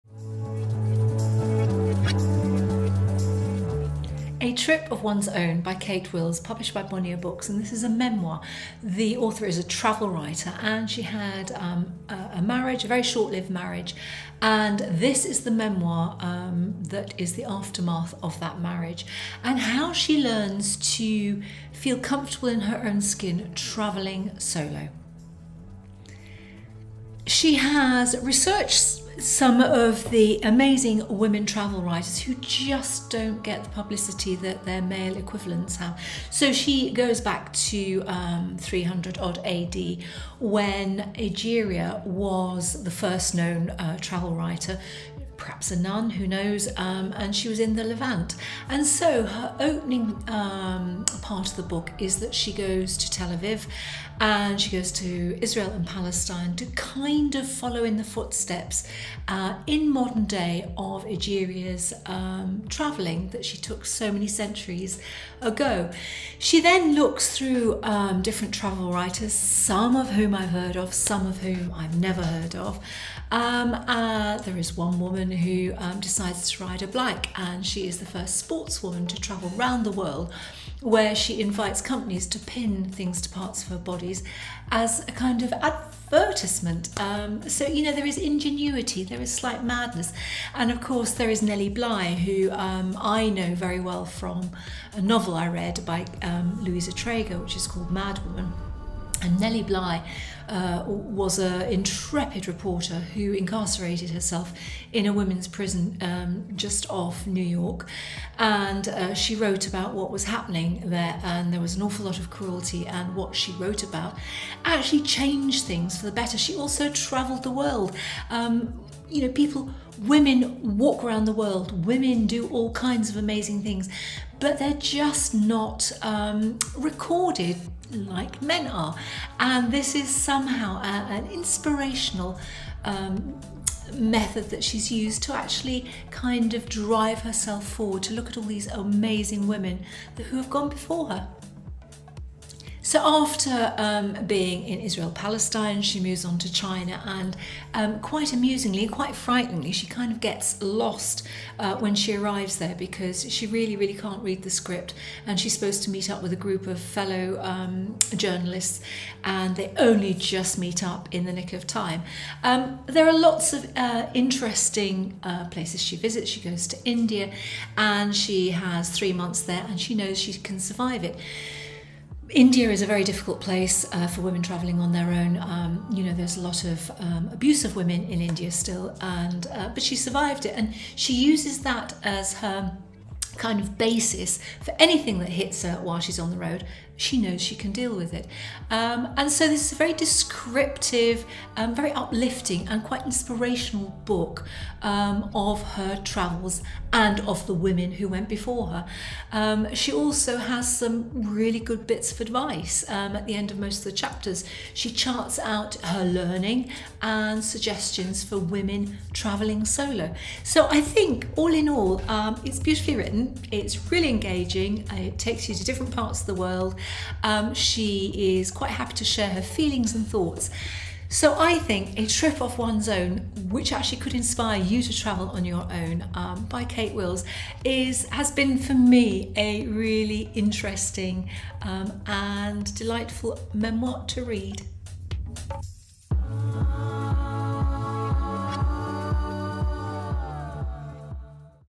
Memoir of solo female travel (A Trip Of One's Own)
A Trip Of One's Own by Kate Wills

Hope, heartbreak and why travelling solo could change your life

Find out about solo female travellers of yore, they don't get nearly as much kudos as men. And from there the author builds up her own memoir of travelling on her own after the ending of her marriage.

It is delightfully written, with insight and wit, and will take you around the world